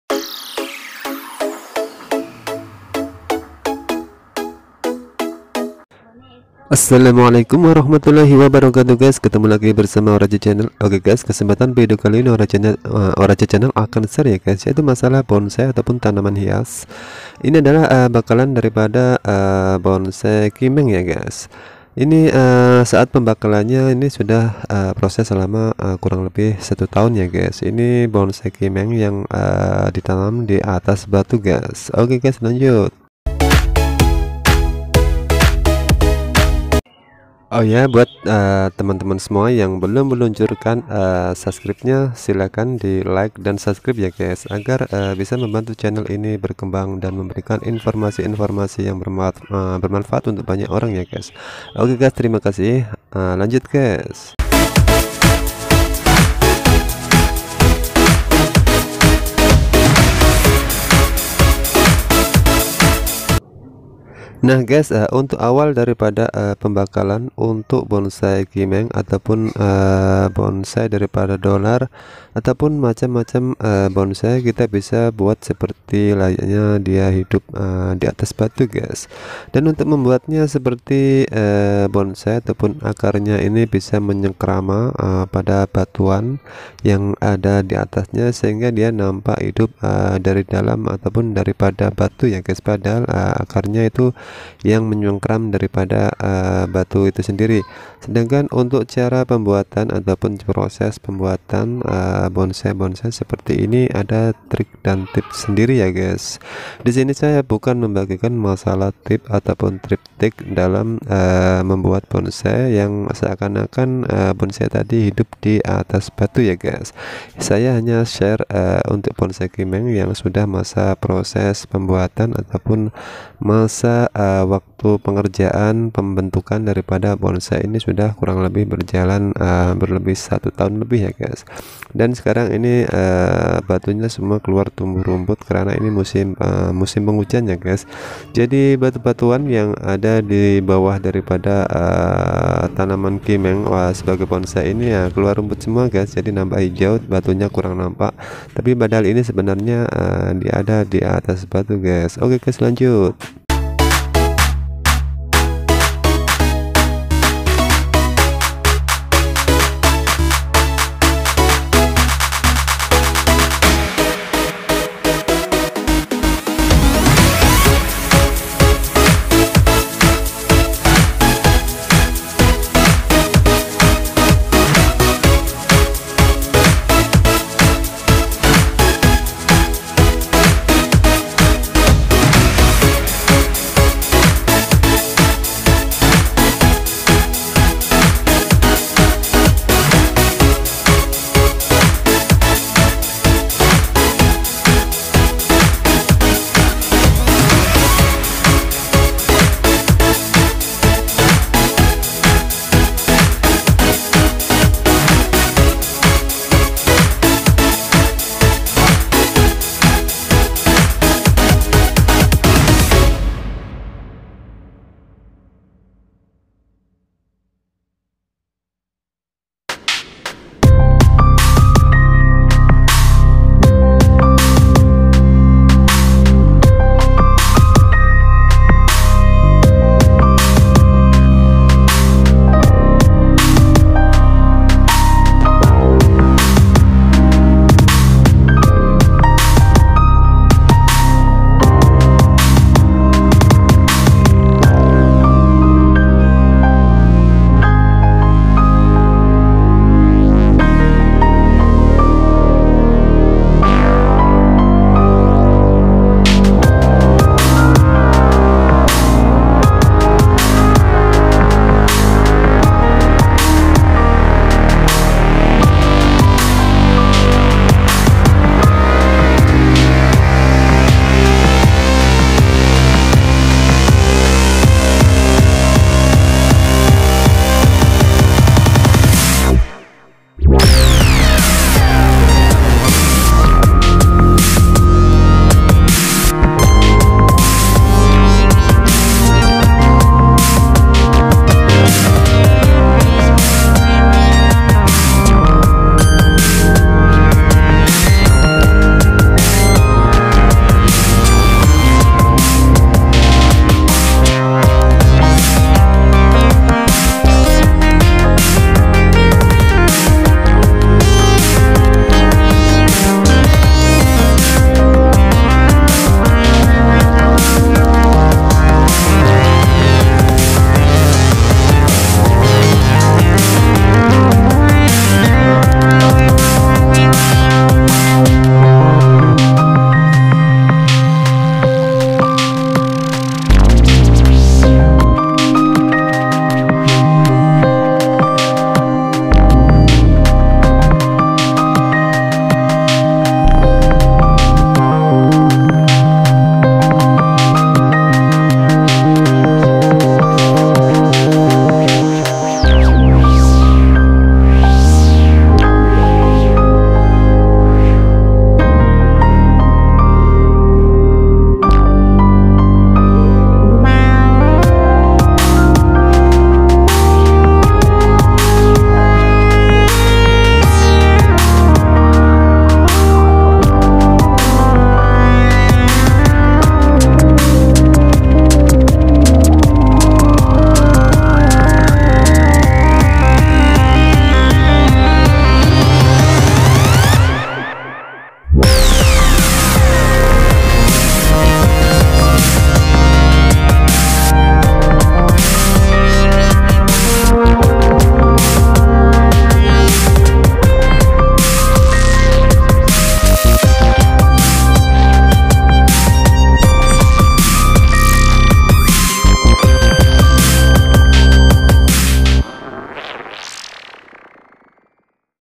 Umur setahun setelah proses tanam atas batu
Bonsai kimeng dari bakalan di tanam atas batu